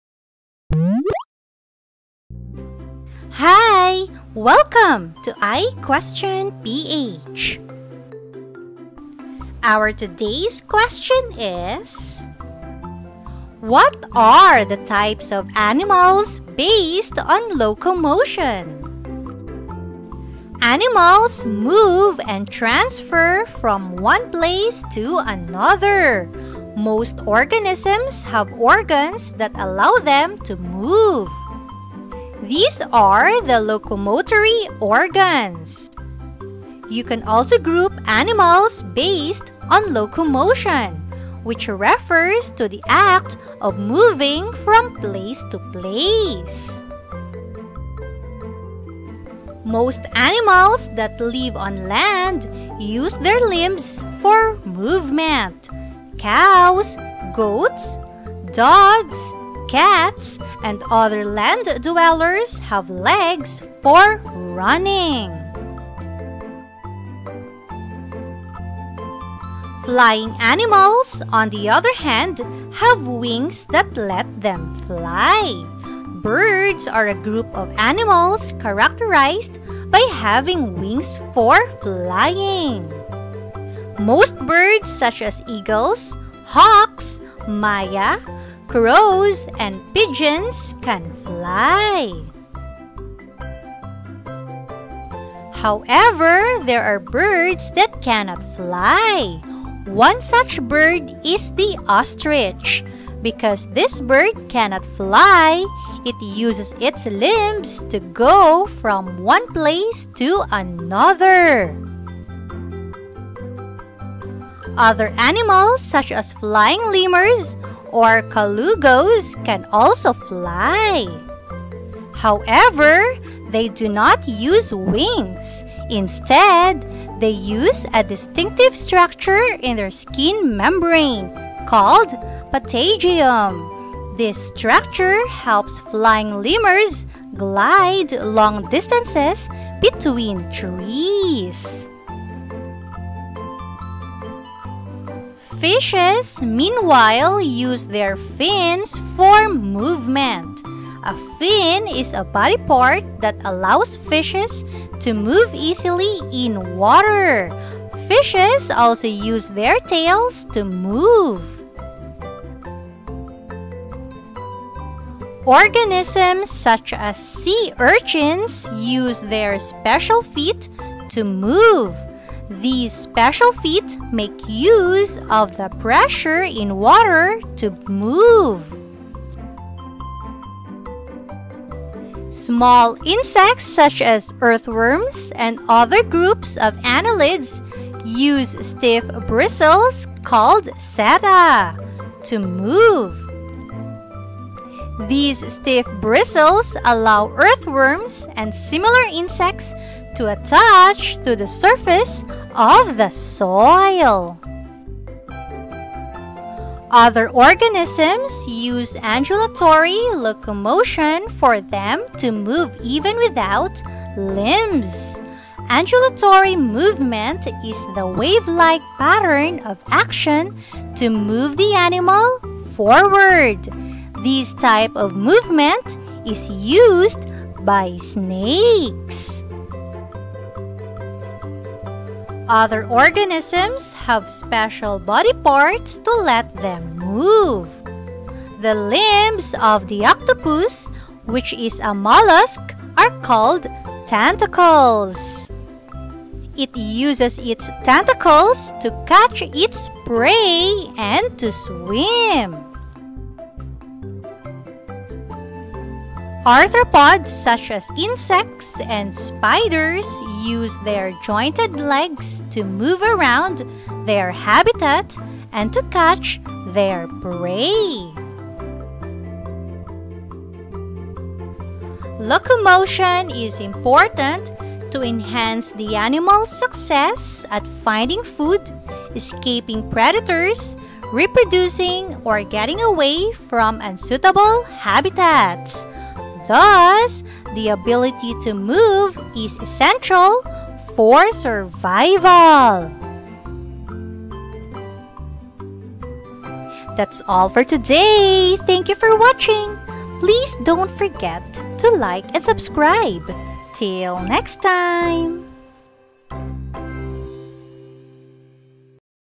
(SCIENCE) What are the Types of Animals Based on Locomotion?
Our today's lesson is about The Types of Animals Based on Locomotion.

Animals move and transfer from one place to another. Most organisms have organs that allow them to move. These are the locomotory organs. You can also group animals based on locomotion, which refers to the act of moving from place to place.

Do you want to learn the types of animals based on their food?